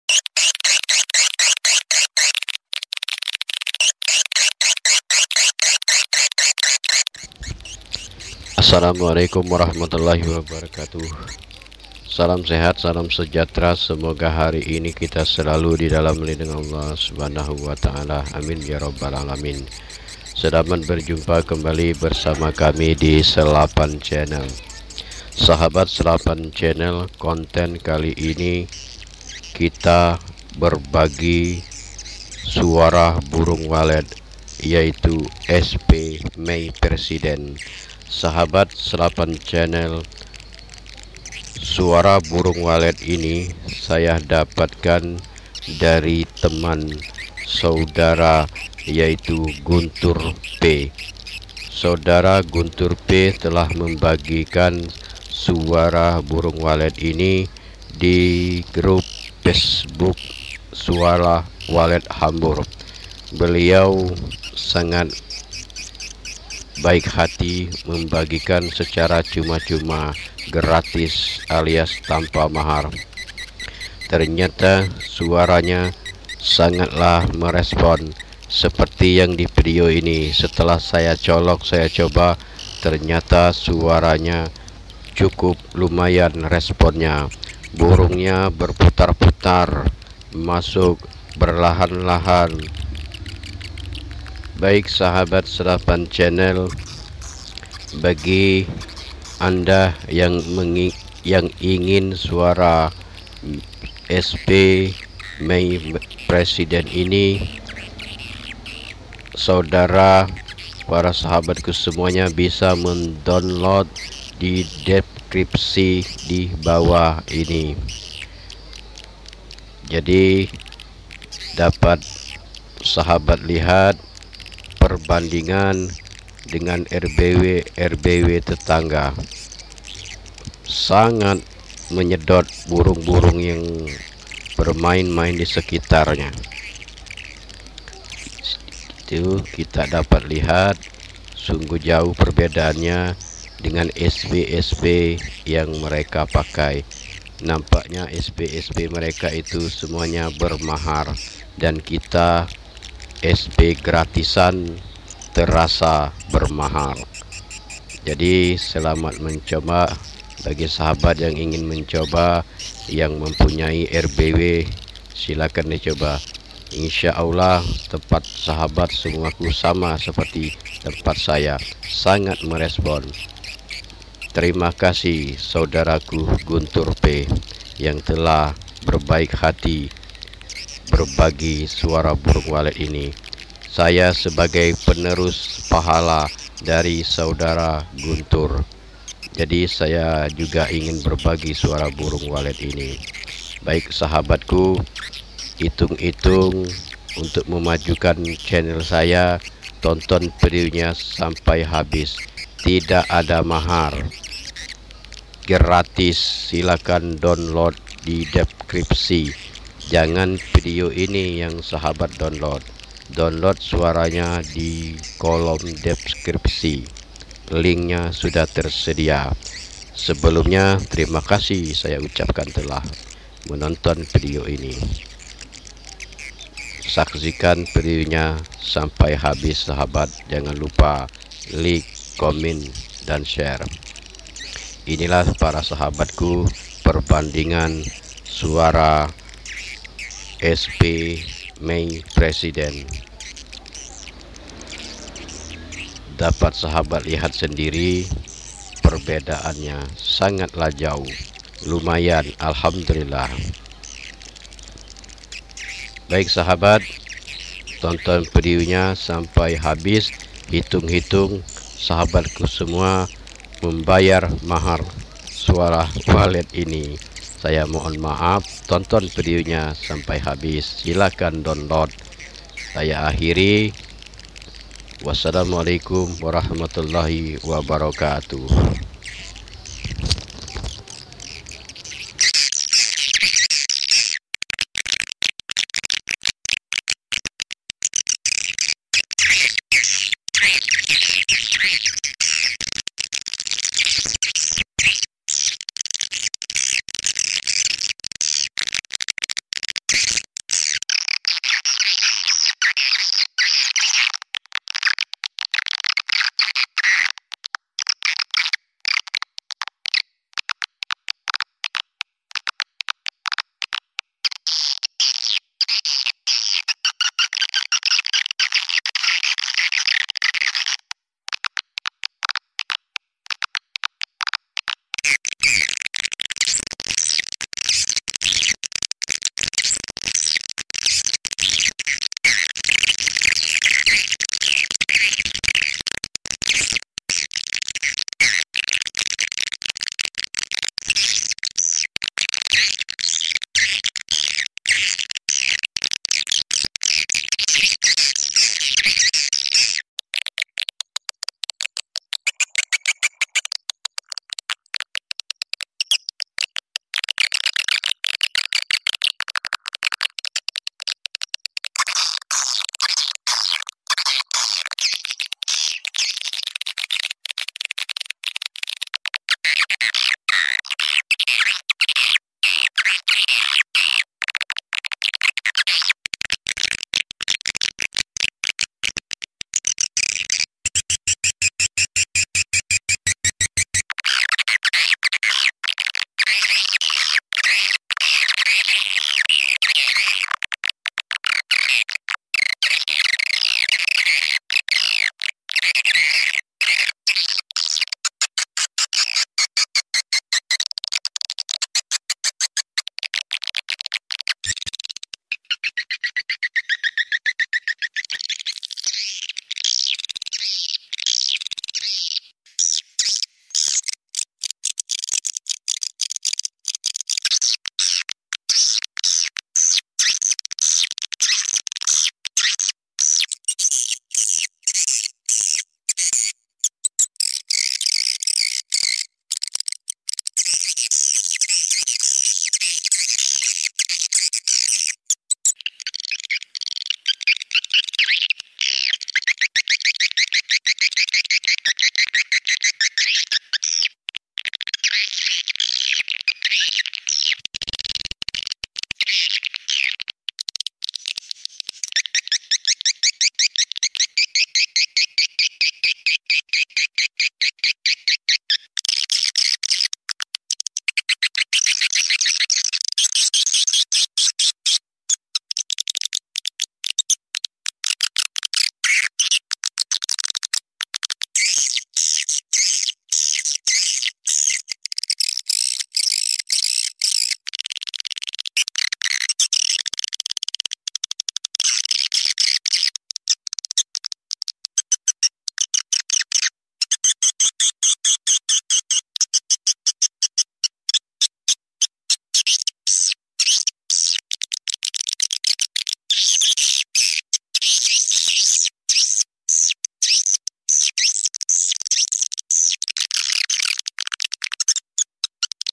Suara panggil burung walet || Sp my president || Amal jariyah Guntur p. Download gratis.
Assalamualaikum warohmatullahi wabarokatu.
apa kabar sahabat smua?
vidio kali ini berbagi suara panggil burung walet yang kami pakai saat ini dan Alhamdulillah sangat merespon.

silakan tonton videonya sampai habis sahabat ku smuanya hitung hitung bayar mahar nya😀🙏

silakan download👇👇
oh yah jangan lupa: